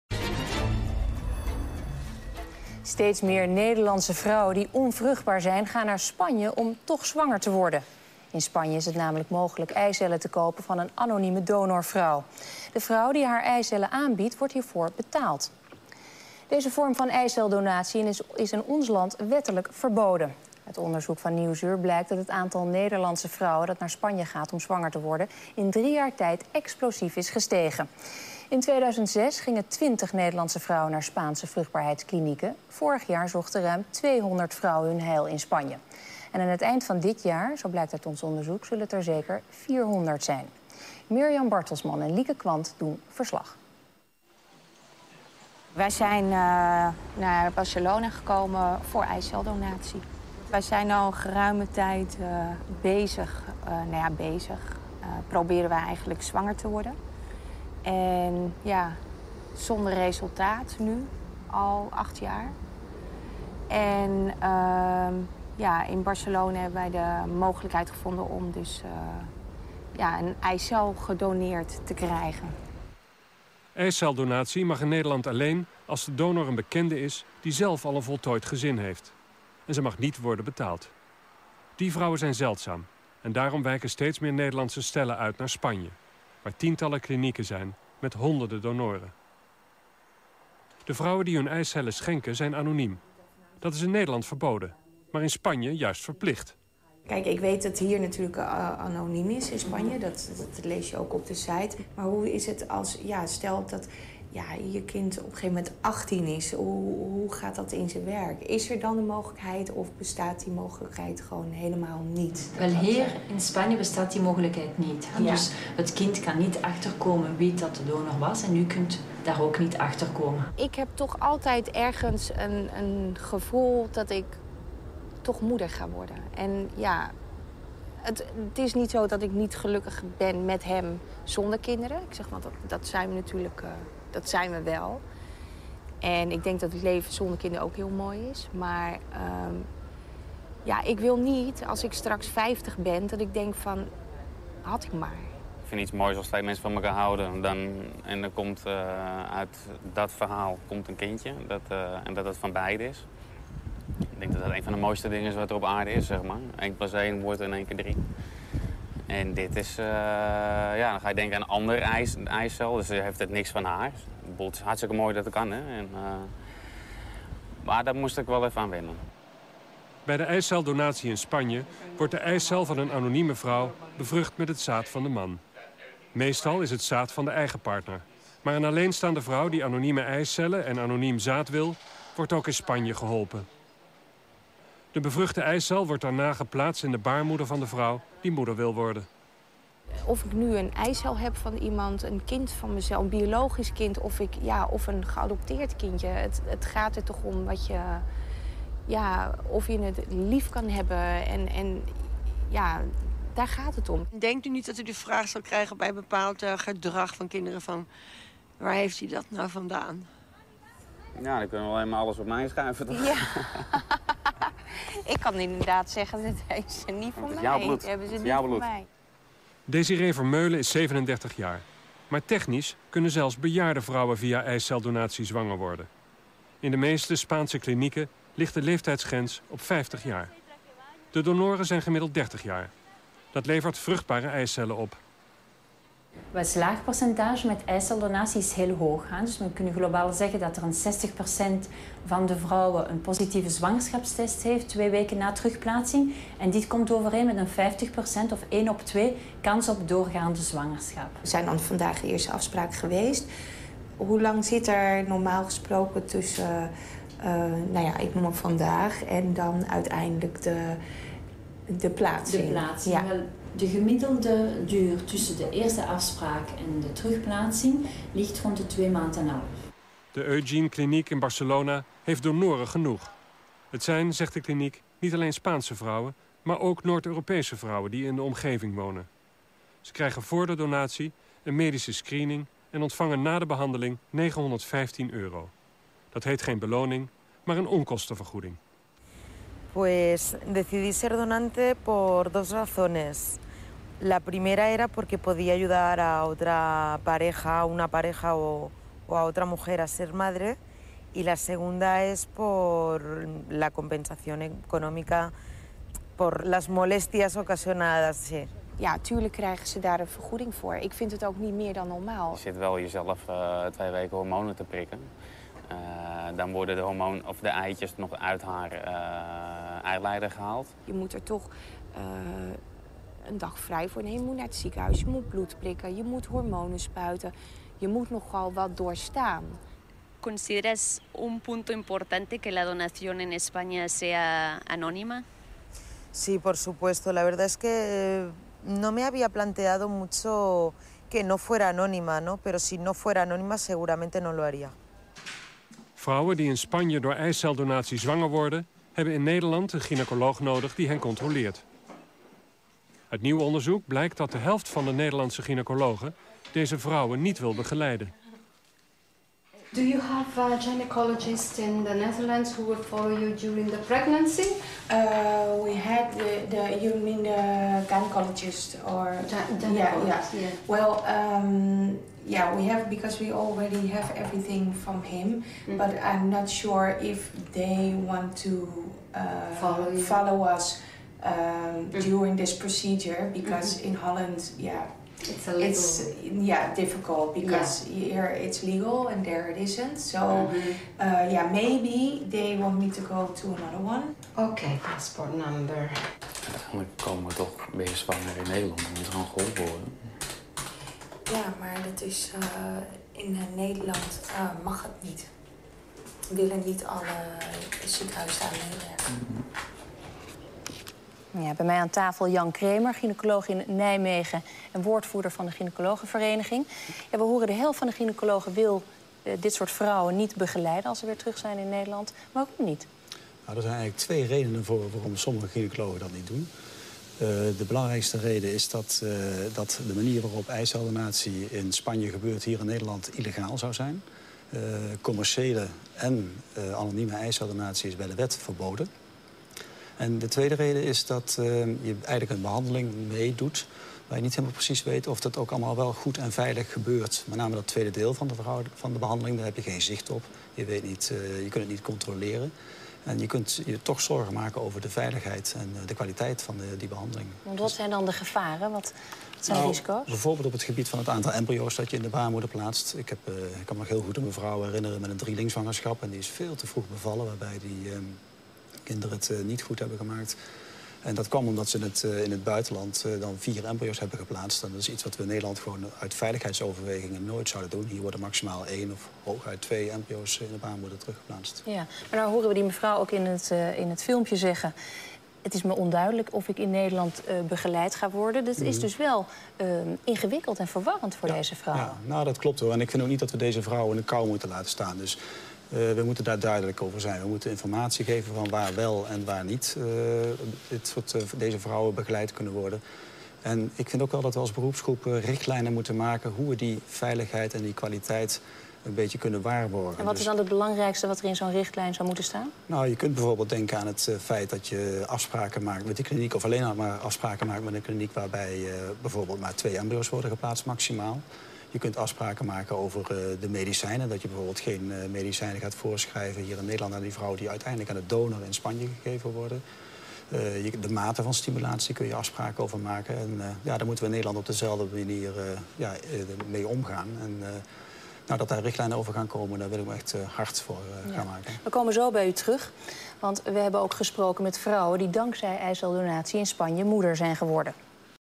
Programa Televisión Holandesa
Programa de la Televisión Holandesa sobre las parejas que vienen a España a someterse a un tratamiento contra la infertilidad.